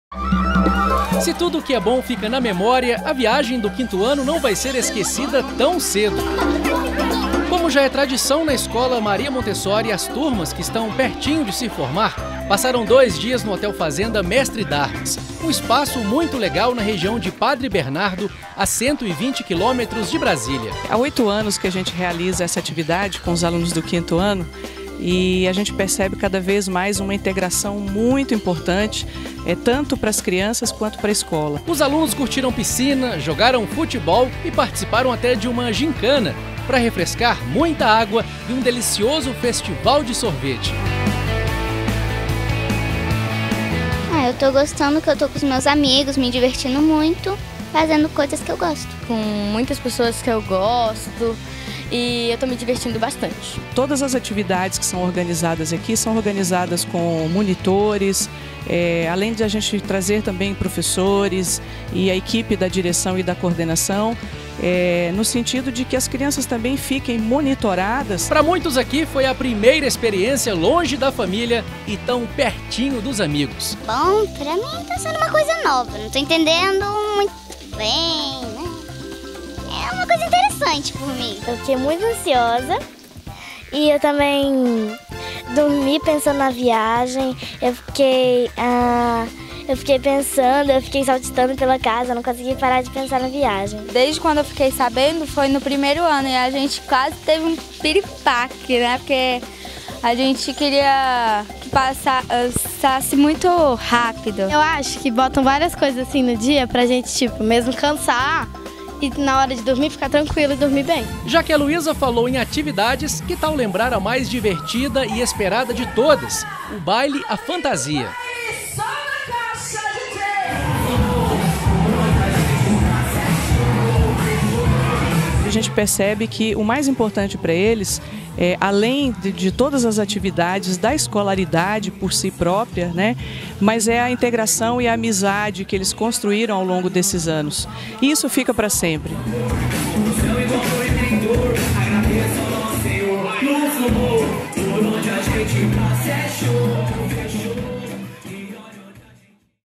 VIAGEM MESTRE D'ARMAS
O assunto de hoje da Tv Montessori é a viagem dos alunos do 5º ano ao Hotel Fazenda Mestre D'Armas, em Padre Bernardo-GO. Diversão pra ficar na memória de todo mundo.